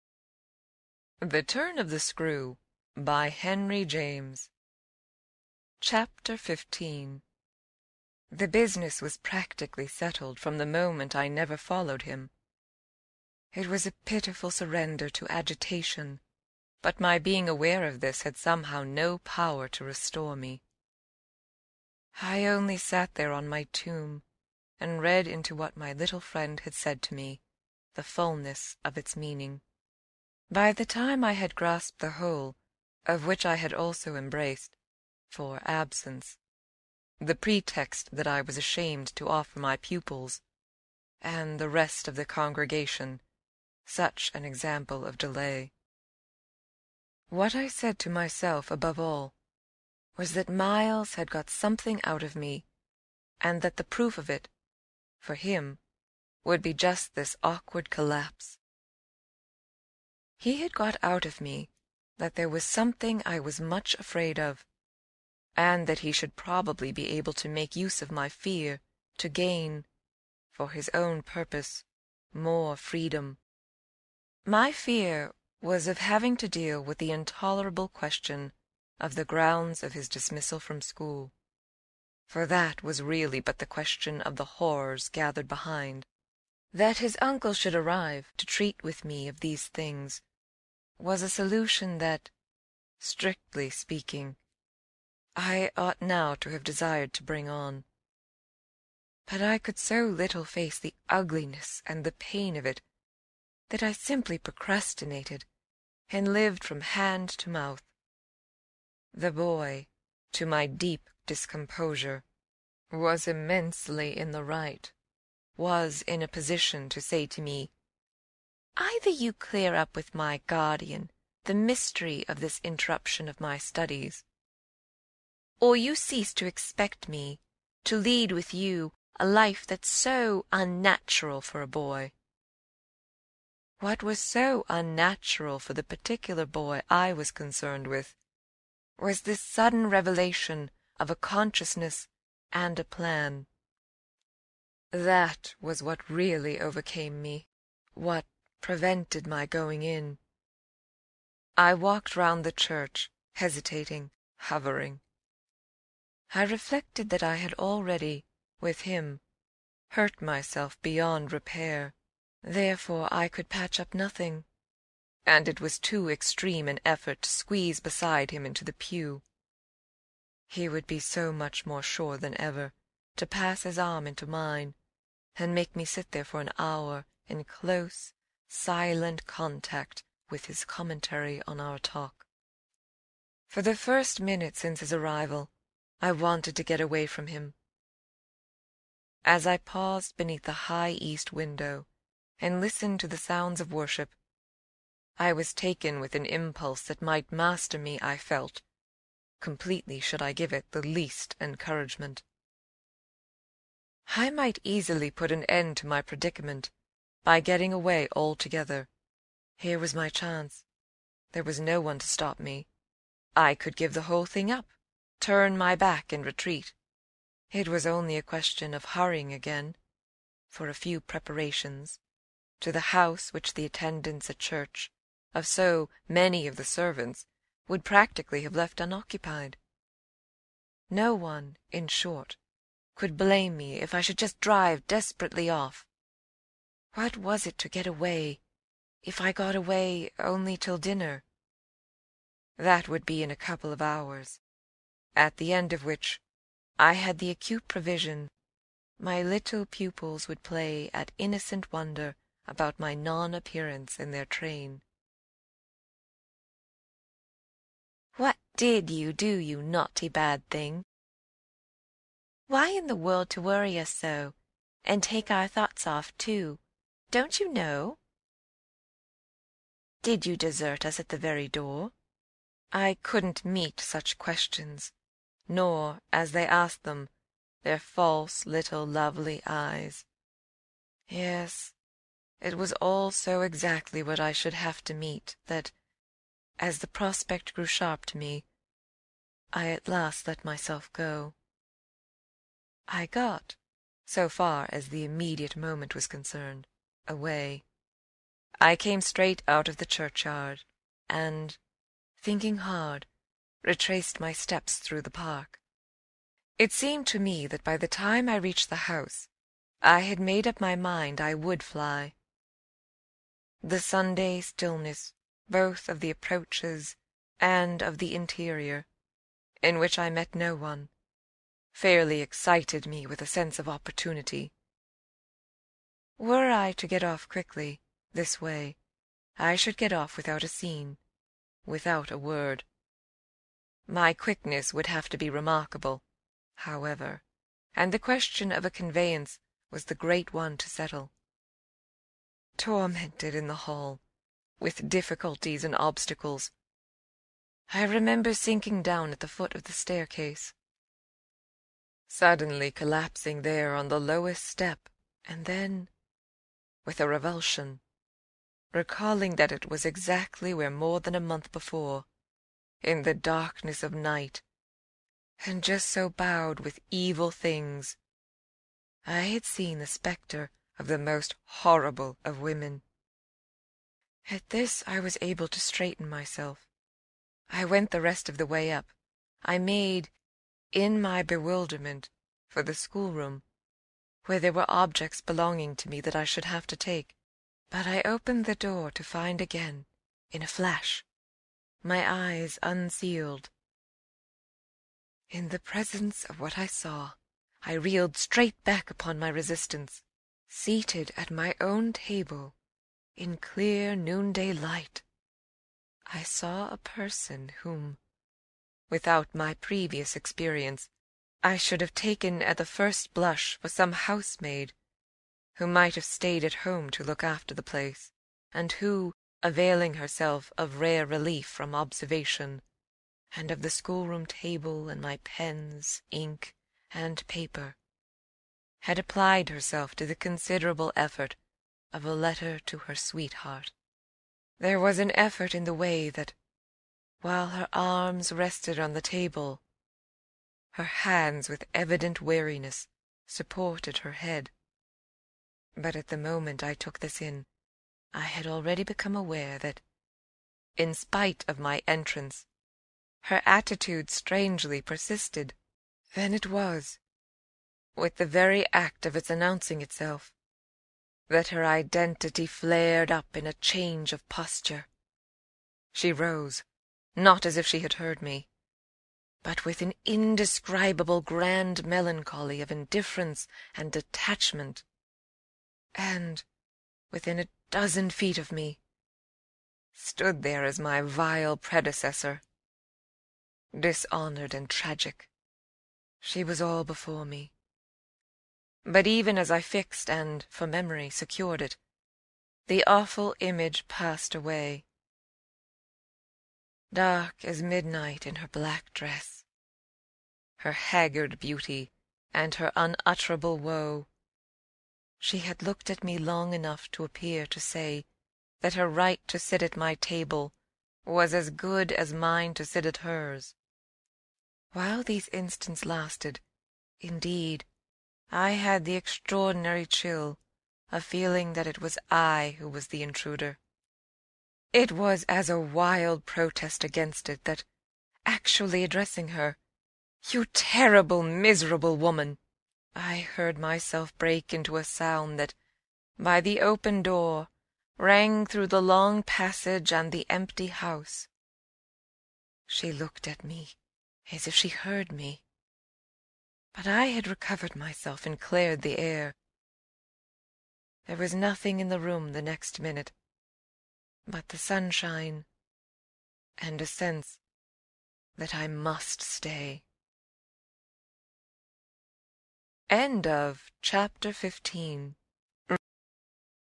Chapter 15 - The Turn of the Screw by Henry James
Chapter 15. Classic Literature VideoBook with synchronized text, interactive transcript, and closed captions in multiple languages. Audio courtesy of Librivox. Read by Nikolle Doolin.